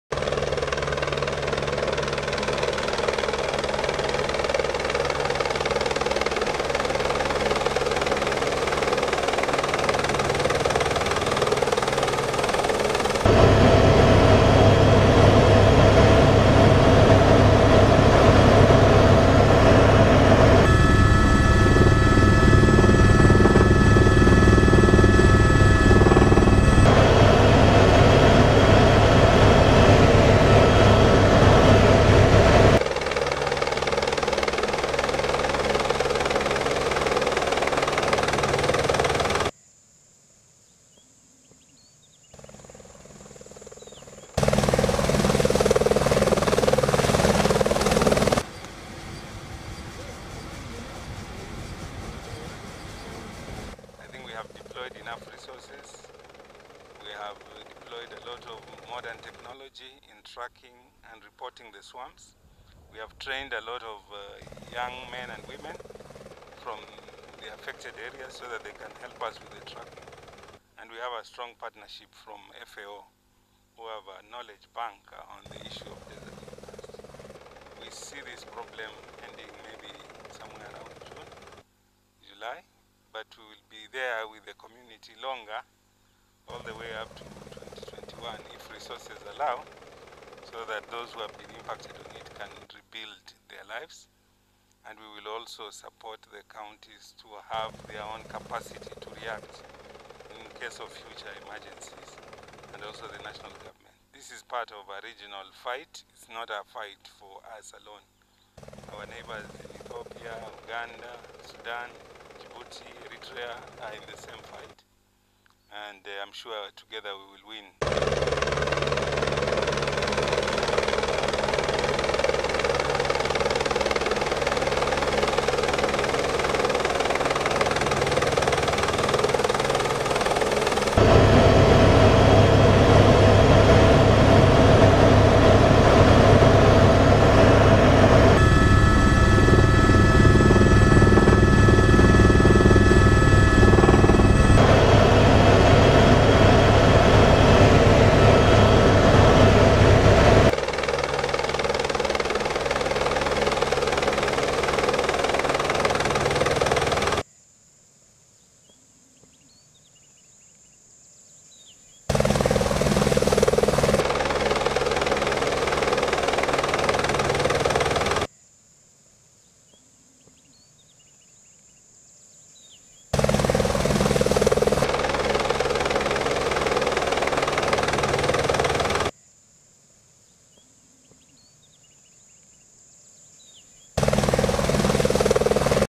Government to wipe out Desert Locusts in June 2020 ... PS Amadi Mboga
Mohoro Daily is a You Tube channel for Breaking News and Developing News.